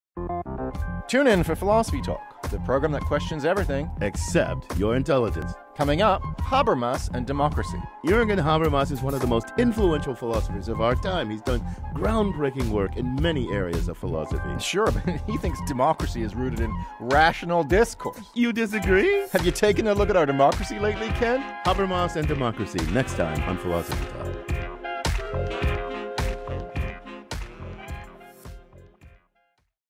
Habermas and Democracy
Jürgen Habermas is regarded as one of the last great public intellectuals of Europe and a major contributor to the philosophy of democracy. A member of the Frankfurt School, Habermas argues that humans can have rational communication that will lead to the democratization of society and consensus. But should we be so optimistic? Why does Habermas have faith in our ability to establish this so-called rational communication and to reach consensus? And how should we reform our liberal democracies to make them more democratic? John and Ken reach for consensus with Matthew Specter from Central Connecticut State University, author of "Habermas: An Intellectual Biography."